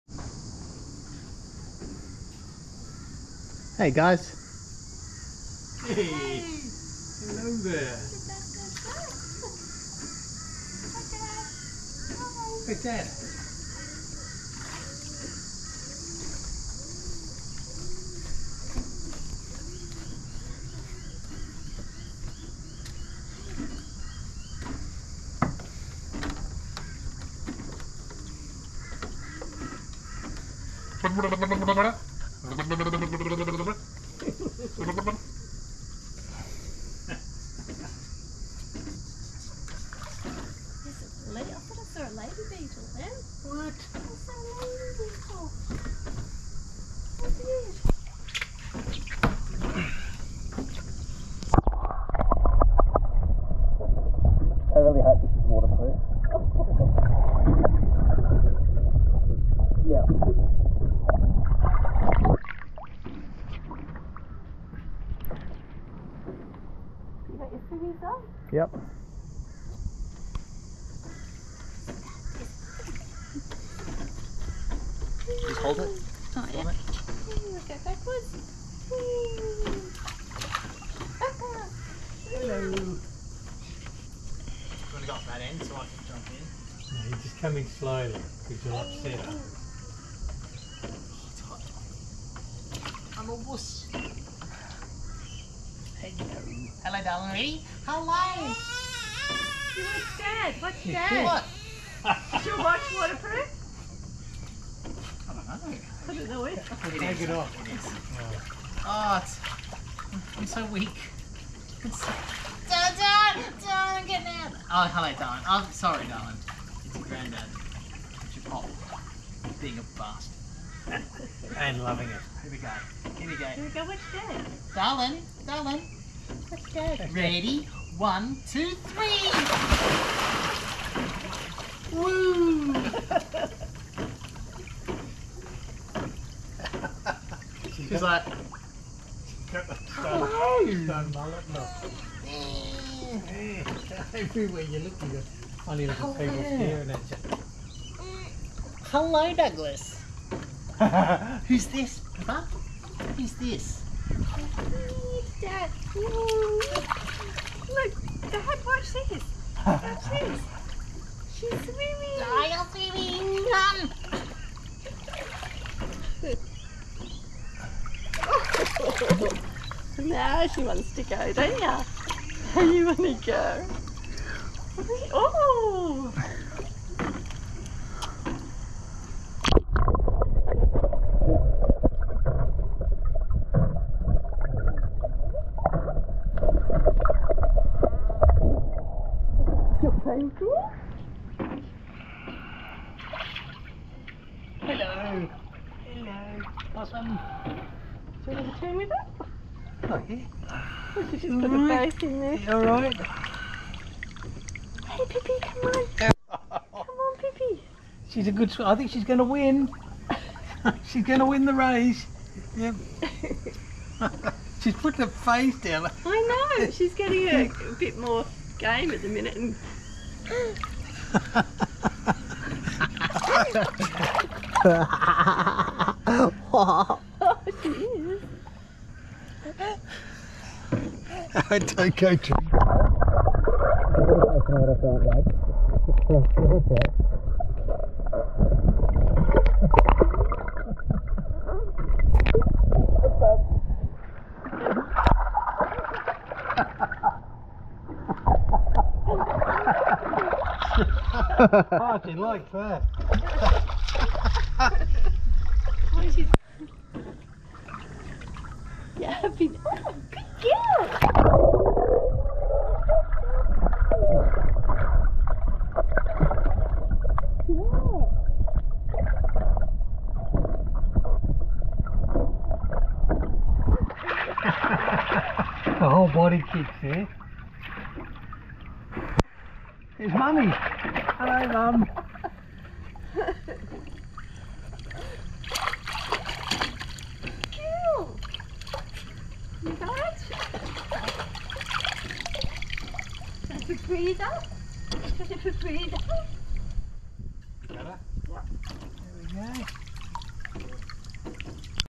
My 35th Birthday with Pip by the Pool
This is the first time the GoPro Hero 10 Black went into the pool, and is one of the very first things I recorded with it. It's December 20th, 2021 - my birthday - and Pip is 7MO and 20 days here (and already loving the water).